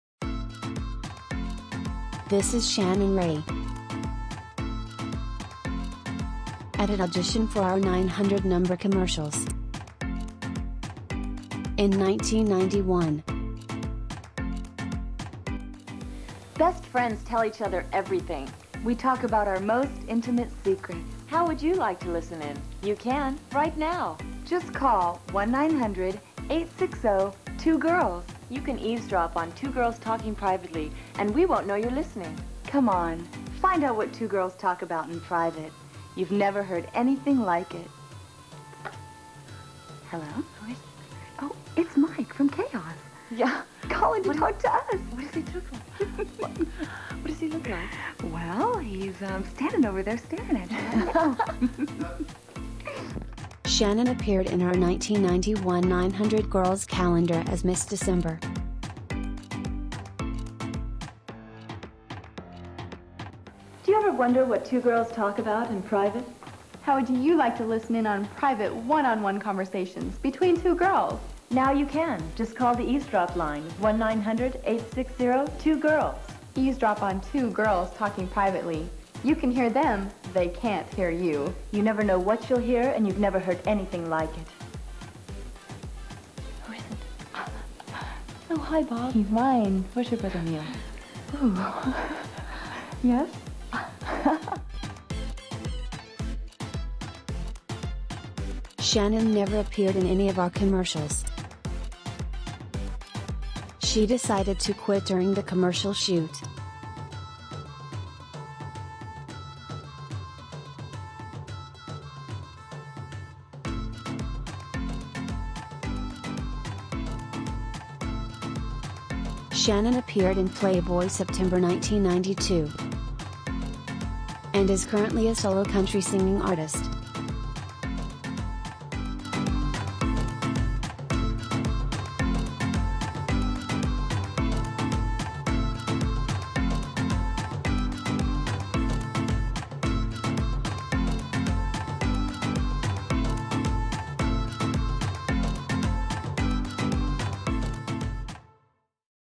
She Did NOT Appear in Our 900-Number TV Commercials (1991) | But She DID Appear in Playboy
I thought this footage from 1991 was lost forever. But wait! I found a very low-quality copy, full of dust and almost unusable. Let's take a look at it anyway. It'll be worth it.

This is Shannon Rae. When she auditioned for our 900-number commercials she was a model with aspirations of a singing career. She was a very attractive, talented, and a nice person with a great attitude.

Shannon was chosen as a national spokesmodel for Coors Beer, appeared in numerous swimwear and lingerie layouts, was Miss Hot Rod Magazine, appeared in Playboy September 1992, she was a 1994 Bench Warmer Non-Sport, and she had an appearance on the Jay Leno Show. Shannon is also known for her work on Exterminator 2 (1984) and Prison Life (2000).

We thought that we hit the jackpot. Shannon would be a star in our TV commercials. Not so fast. During the commercial shoot, she decided that she no longer wanted to appear in the commercials, packed up, and left. Just like that. Very disappointing. Shannon's ads were never aired.

And now? Shannon is a solo country singing artist. I guess that's what she always wanted to be.

And she did a commercial spot with Kristi Ducati, just like the one with Kristi and Playmate Neriah Davis (neither of the spots aired).